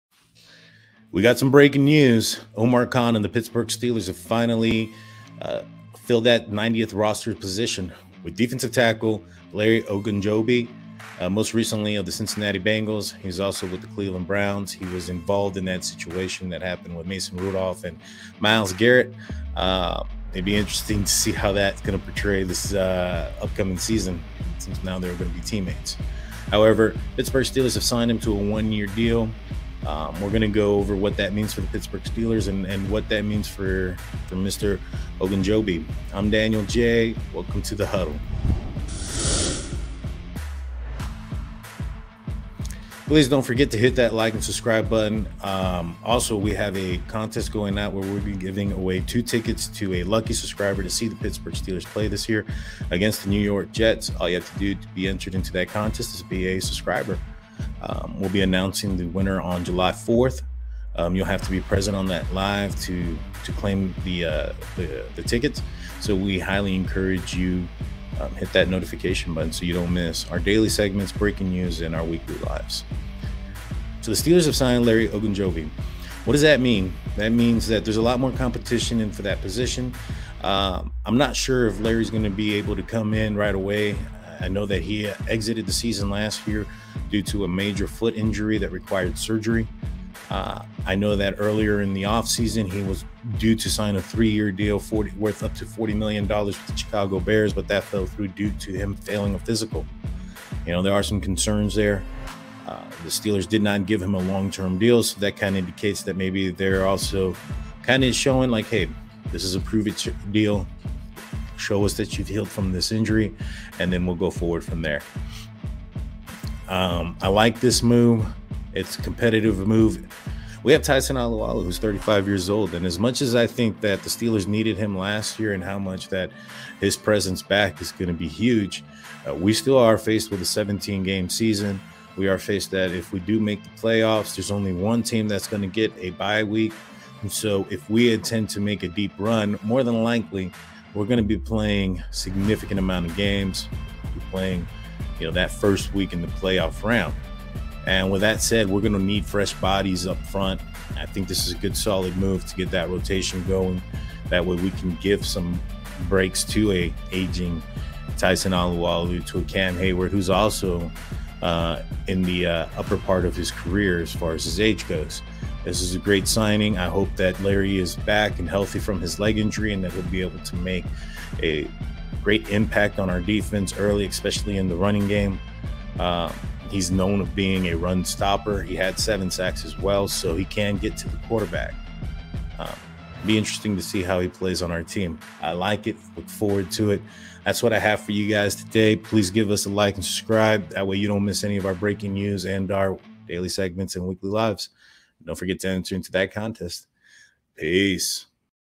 State of the Steelers Breaking News Steelers Sign Dt Larry Ogunjobi
Steelers herewego Steelers General Manager Omar Kahn makes next big move signing dt larry Ogunjobi